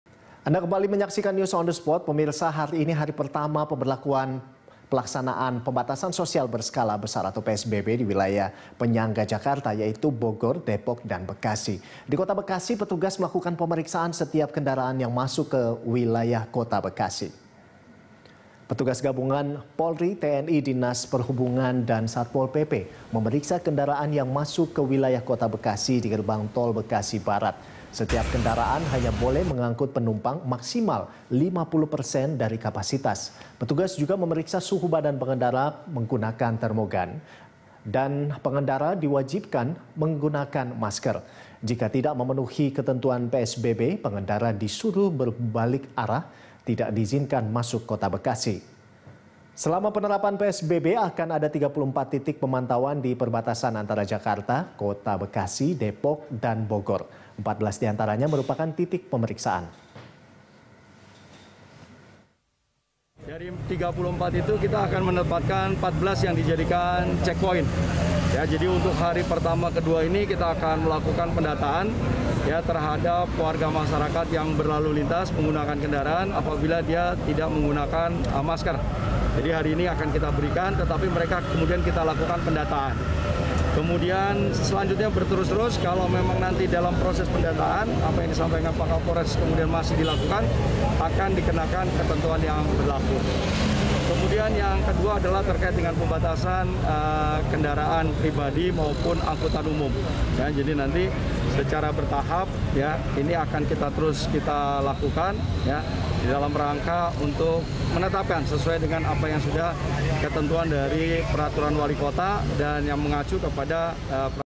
Hari Pertama PSBB di Bekasi
Hari ini, hari pertama pemberlakuan PSBB di wilayah penyangga Jakarta yaitu Bogor, Depok dan Bekasi. Di Kota Bekasi, petugas melakukan pemeriksaan setiap kendaraan yang masuk ke wilayah Kota Bekasi.